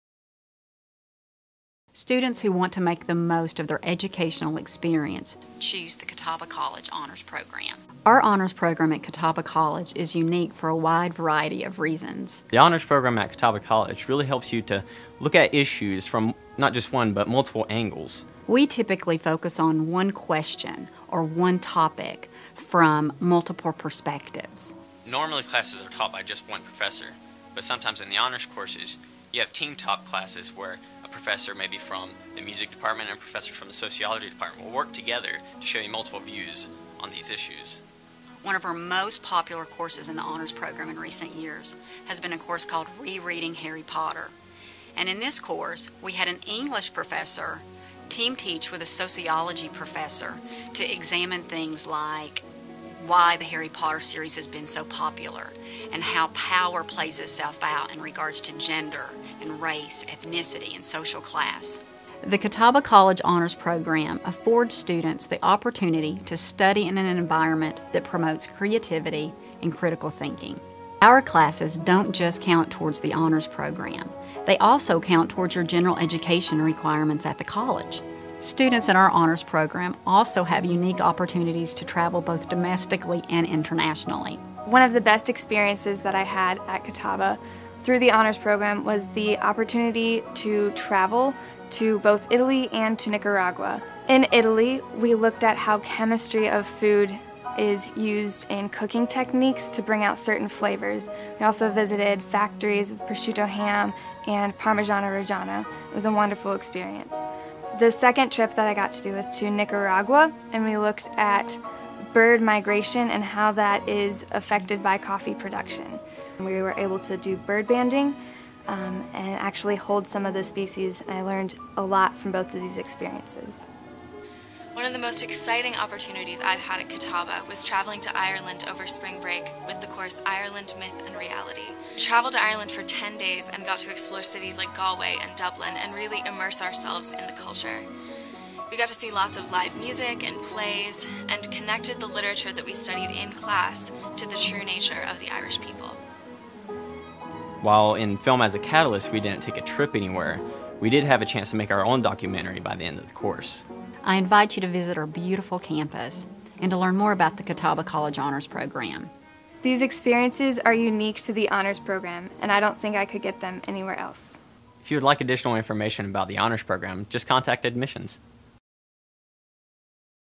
Honors Program at Catawba College
Students who want to make the most of their educational experience choose the Catawba College Honors Program.

Our Honors program is unique for a wide variety of reasons. Classes are often team-taught by faculty members from two entirely different disciplines, often focusing on one question or topic from multiple perspectives.

Students study in an environment that promotes creativity and critical thinking and the courses count toward your general education requirements for the college.

Students also have the opportunity to travel both domestically and internationally (i.e. Italy, Nicaragua, Jamaica, Ireland, Atlanta, Monticello, and Chicago).

Music performed by Catawba Artists.